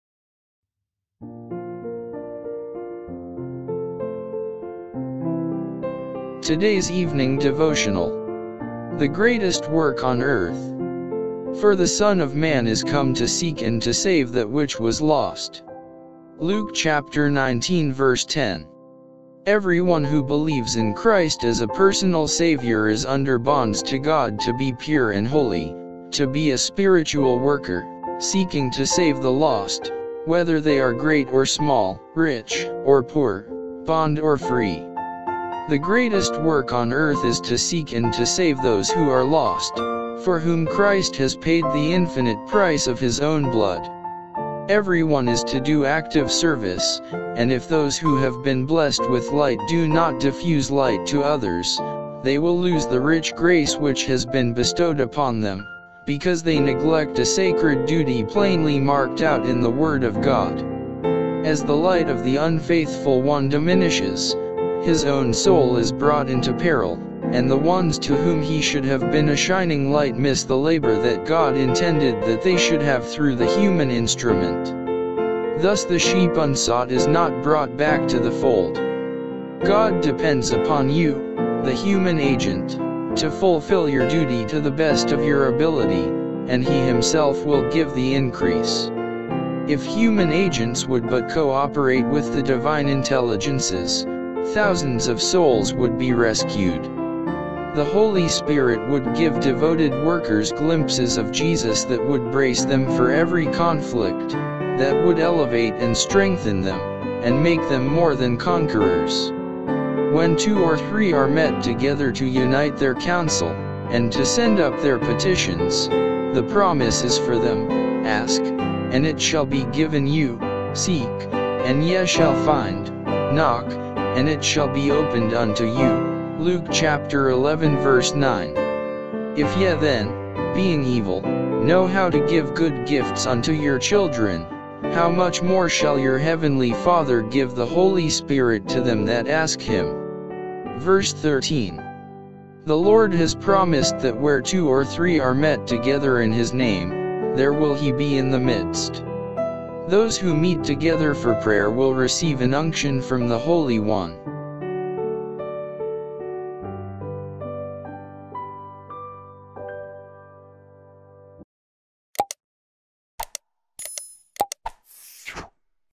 The Greatest Work on Earth - Today's Evening Devotional 06-01-22
Daily Devotion

The Greatest Work on Earth

For the Son of man is come to seek and to save that which was lost. Luke 19:10

Everyone who believes in Christ as a personal Saviour is under bonds to God to be pure and holy, to be a spiritual worker, seeking to save the lost, whether they are great or small, rich or poor, bond or free. The greatest work on earth is to seek and to save those who are lost, for whom Christ has paid the infinite price of His own blood. Everyone is to do active service, and if those who have been blessed with light do not diffuse light to others, they will lose the rich grace which has been bestowed upon them, because they neglect a sacred duty plainly marked out in the Word of God. As the light of the unfaithful one diminishes, his own soul is brought into peril; and the ones to whom he should have been a shining light miss the labor that God intended that they should have through the human instrument. Thus the sheep unsought is not brought back to the fold.

God depends upon you, the human agent, to fulfil your duty to the best of your ability, and He Himself will give the increase. If human agents would but cooperate with the divine intelligences, thousands of souls would be rescued. The Holy Spirit would give devoted workers glimpses of Jesus that would brace them for every conflict, that would elevate and strengthen them, and make them more than conquerors. When two or three are met together to unite their counsel, and to send up their petitions, the promise is for them: “Ask, and it shall be given you; seek, and ye shall find; knock, and it shall be opened unto you” (Luke 11:9). “If ye then, being evil, know how to give good gifts unto your children: how much more shall your heavenly Father give the Holy Spirit to them that ask him?” (verse 13). The Lord has promised that where two or three are met together in His name, there will He be in the midst. Those who meet together for prayer will receive an unction from the Holy One.